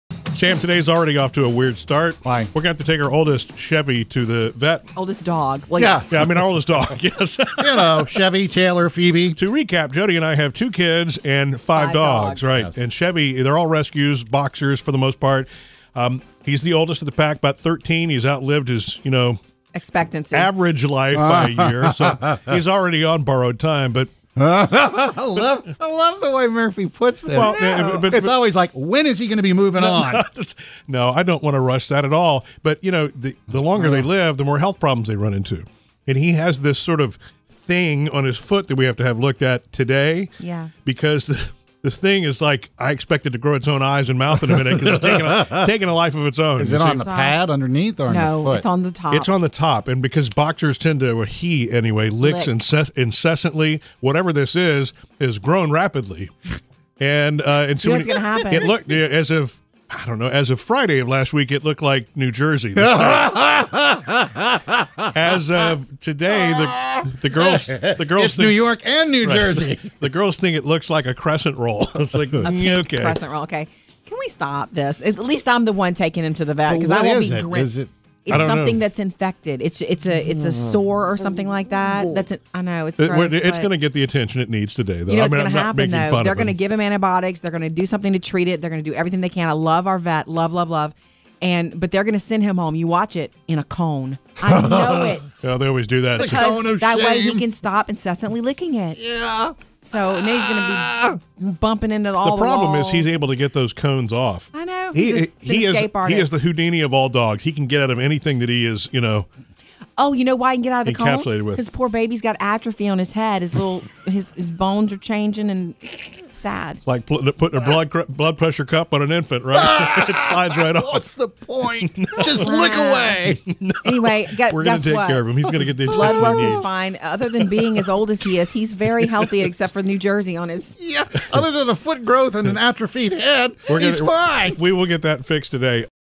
Chevy's Sore
Murphy and Jodi's oldest dog, Chevy, has an interesting looking sore on its foot. They are going to the vet but first they had to tell Sam about it.